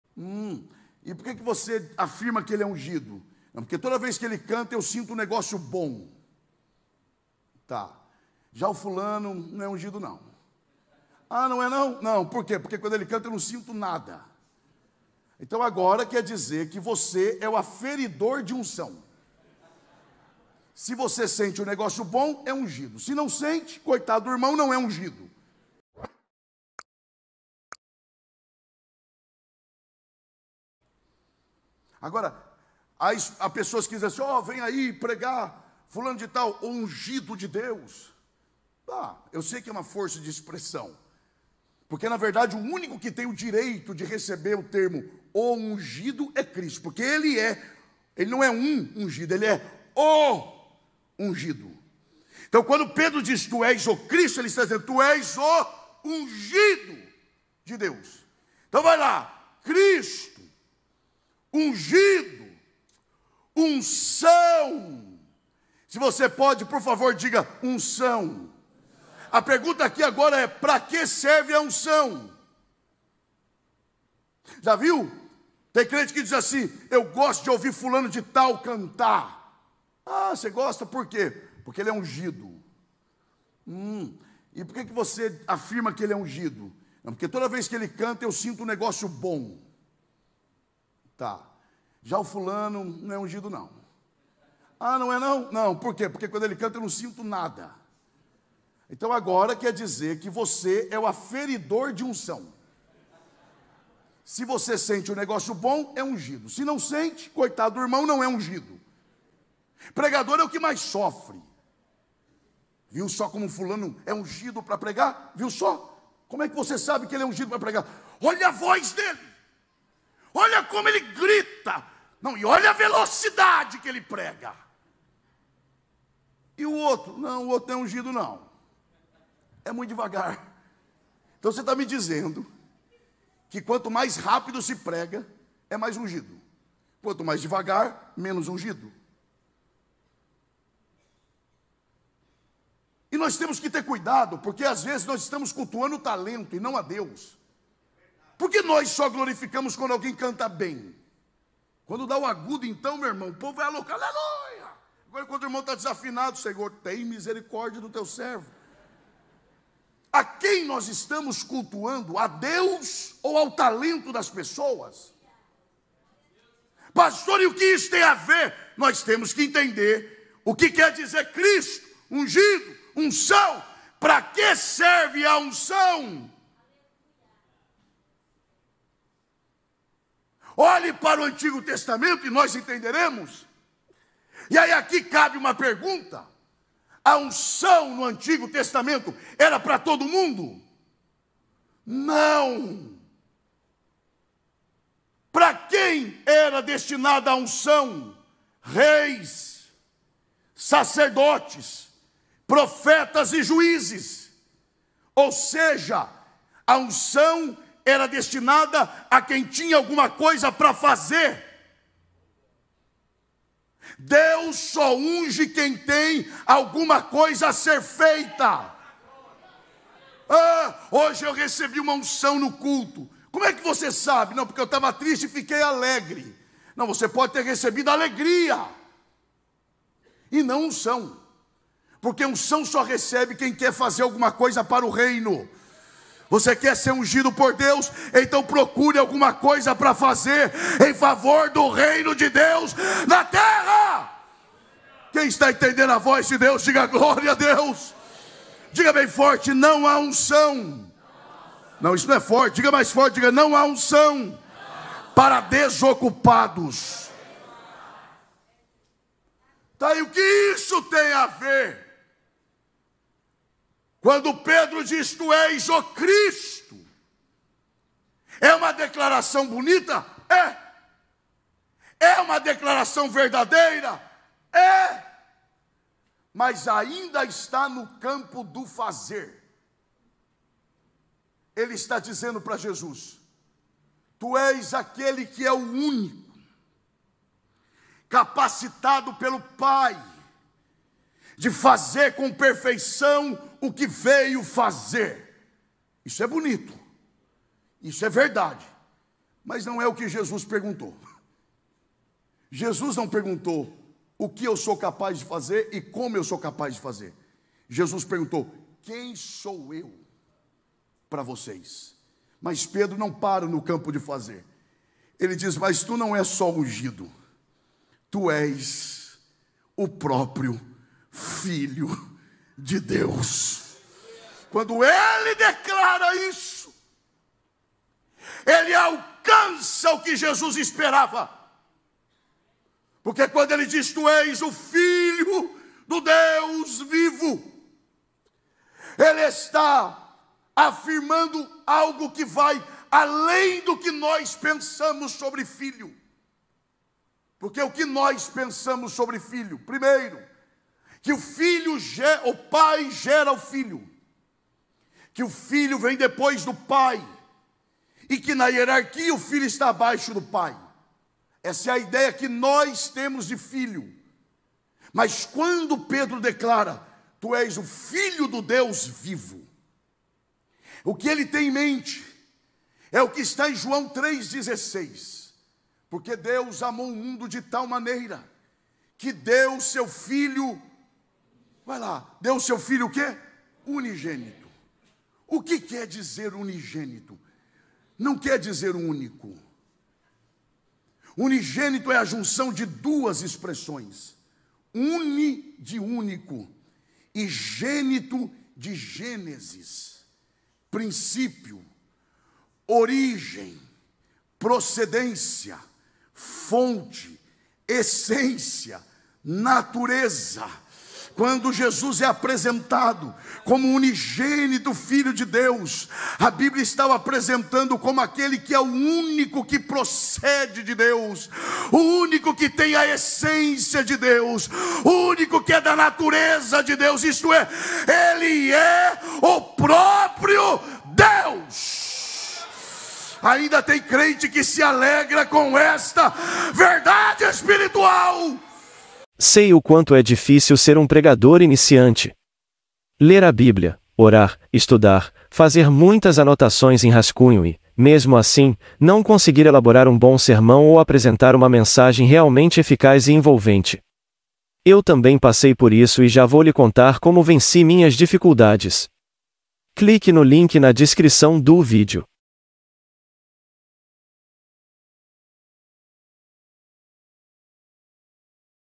VOCÊ SABE MESMO O QUE É UNÇÃO? Pr Elias Torralbo - Edson Borges
Pastor Elias Torralbo nesse vídeo surpreende ao falar que muitos entendem unção de forma equivocada e apresenta de fato o que é ser ungido. deixe seu comentário o que você aprendeu sobre.

link do pastor: @pr.eliastorralbo
link da igreja: @adblu